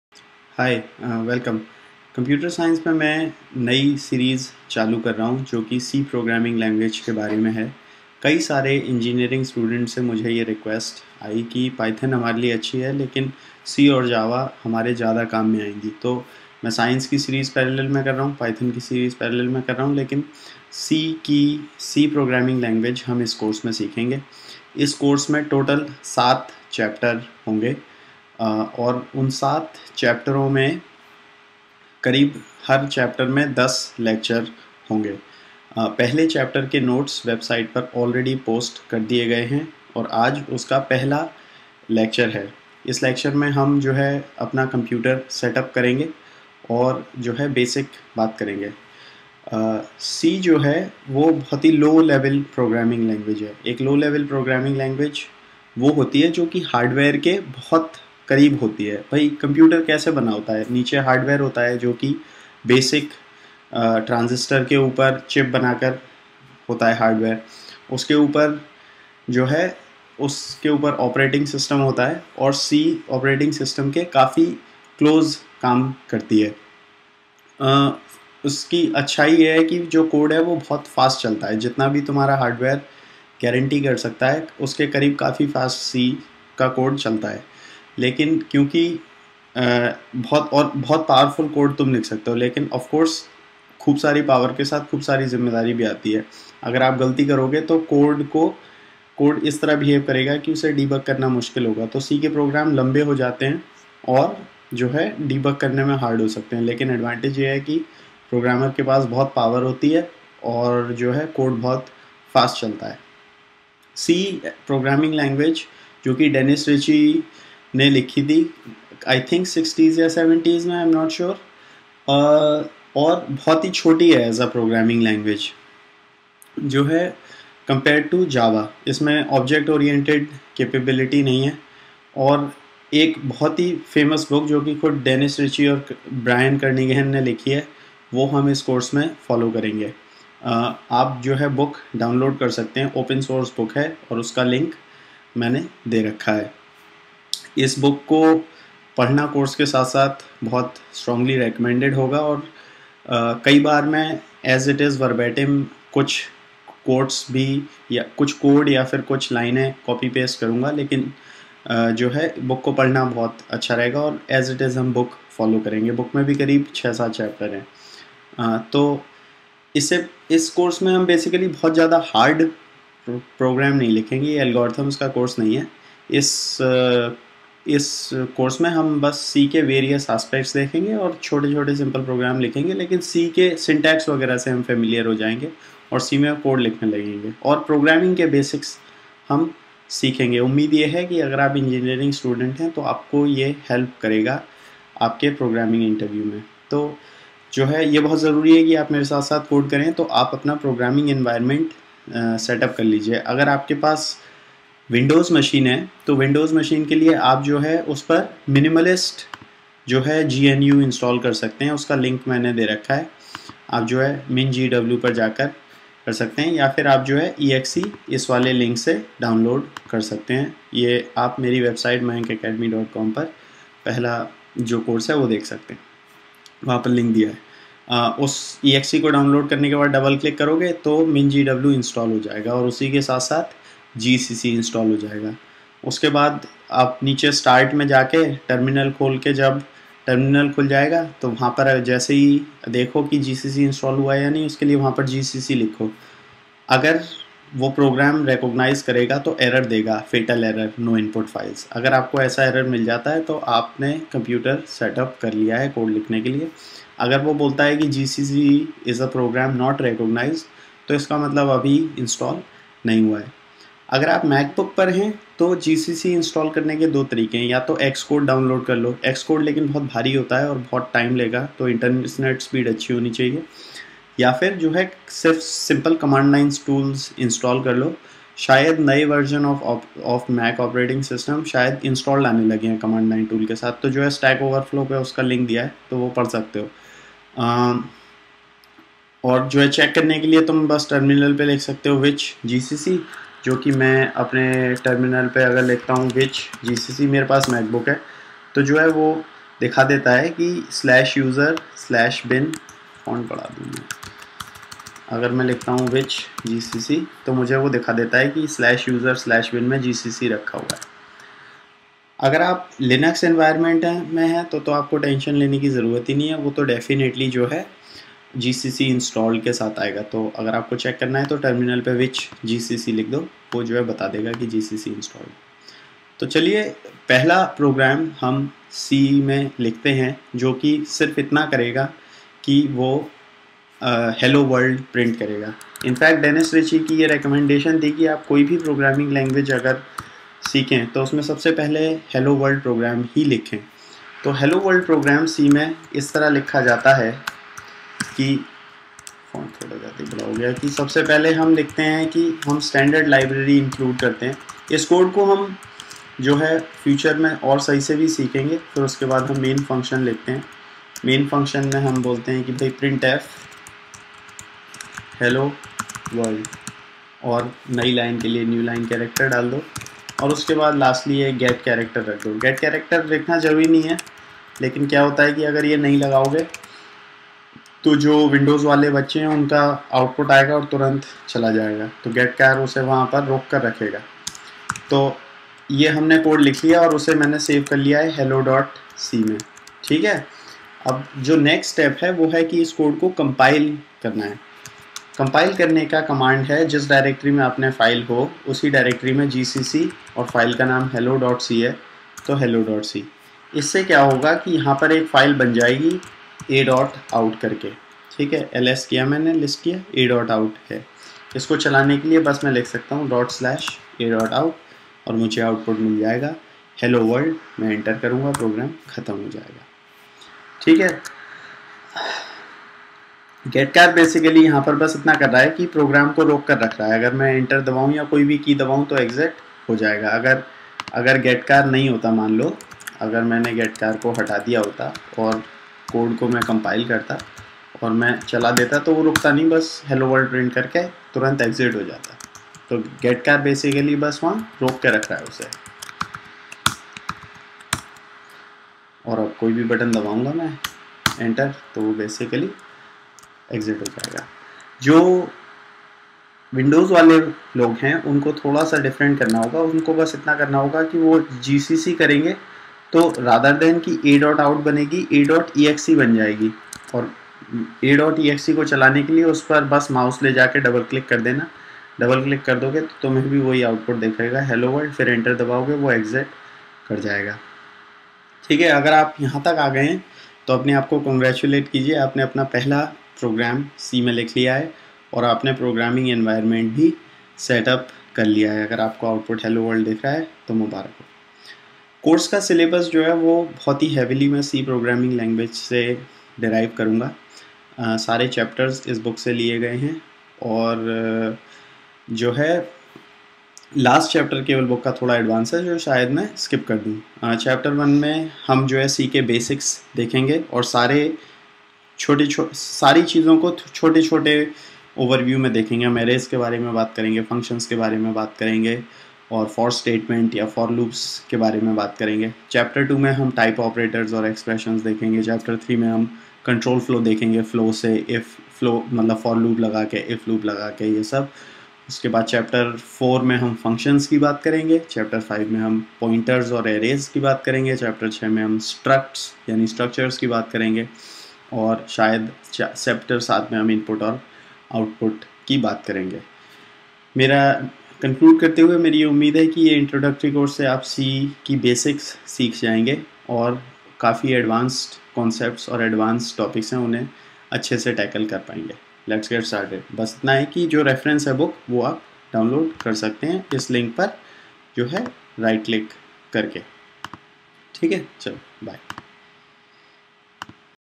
C Programming: Course Introduction and Setup [in Hindi]
In this lectures, I kickstart the course: "Introduction to Programming in C". I provide some instructions on how to setup your computer environment for C, as well as write the hello world program in C.